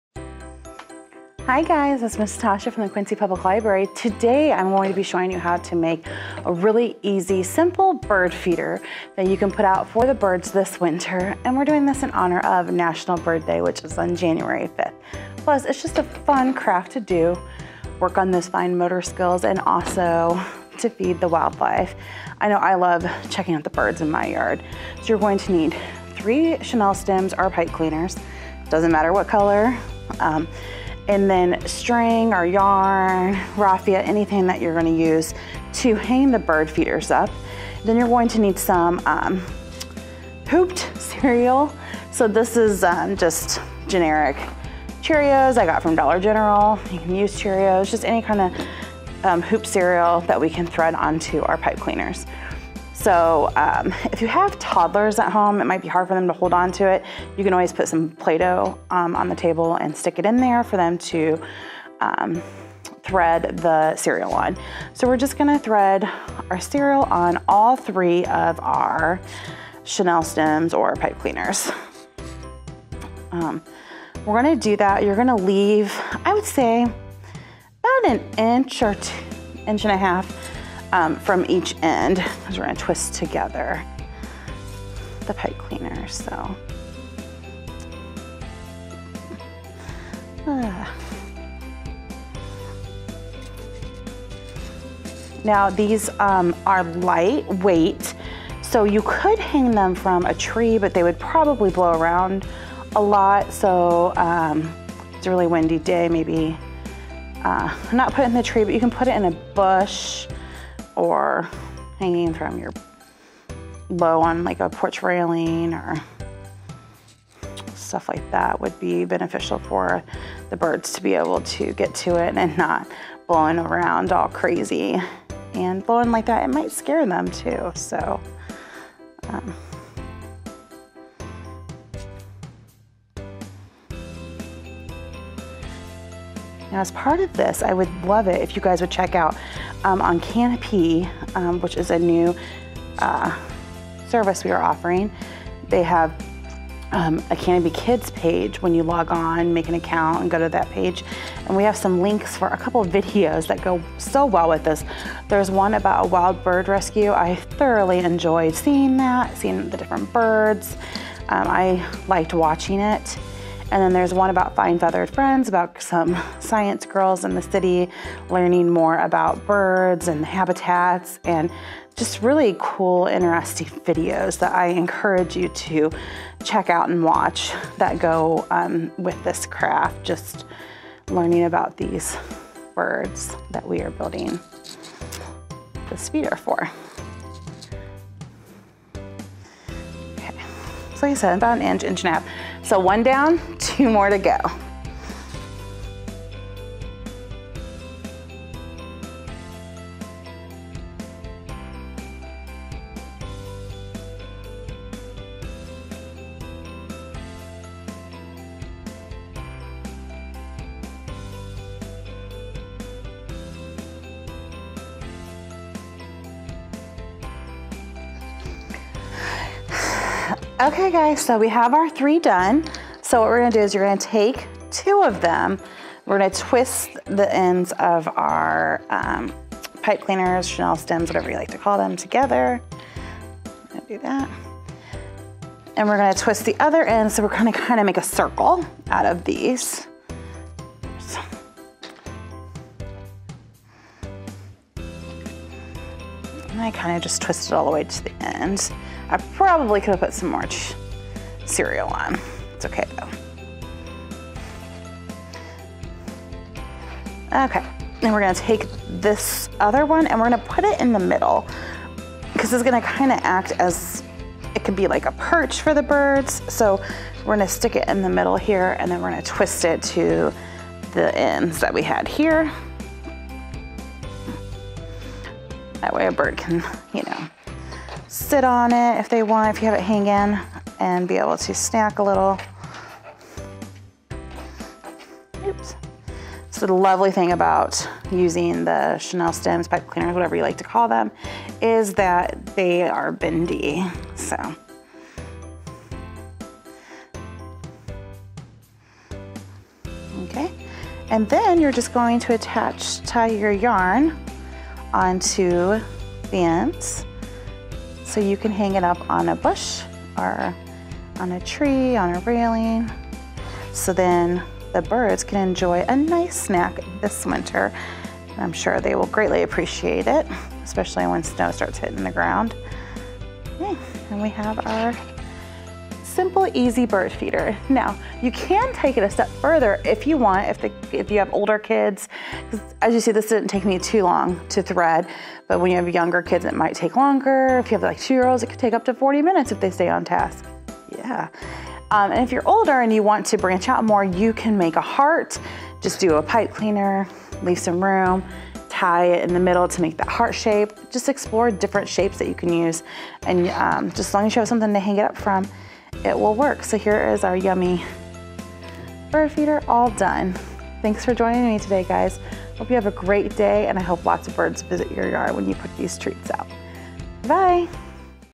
How To Make a Bird Feeder
Join Miss Natascha as she shows you how to create an easy bird feeder just in time to celebrate National Bird Day (January 5)!  The supplies you will need for this craft are Chenille Stems or Pipe Cleaners, Cheerios, and String (raffia, yarn, something to hang your feeder from).

After creating your bird feeder check out these videos about feathered friends on Kanopy using your library card: